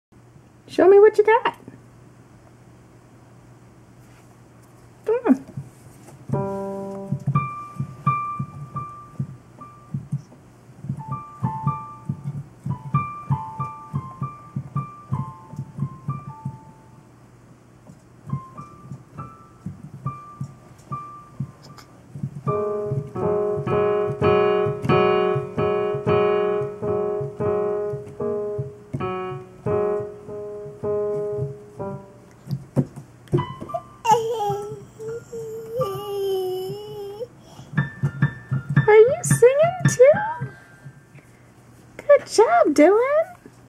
23 month old Sir Dylan Eli practicing piano with hearing aids
Dylan got his hearing aids just over a month ago.  He loves music. I think he's got a knack for it.
Born on Valentine's Day 2012, 11 weeks premature, weighing 3 lbs and battled a dangerous infection for 101 days in the NICU.  He's had 15 brain surgeries to treat the Hydrocephalus he acquired while battling the infection.

In 2013 he developed moderate hearing loss and in December he got hearing aids and loves them!

He's been through a lot in his 23 months but his joy is infectious!

If you wish to contact Dylan Lipton-Lesser's Mamma Indy, find her on facebook under India Elizabeth Lipton.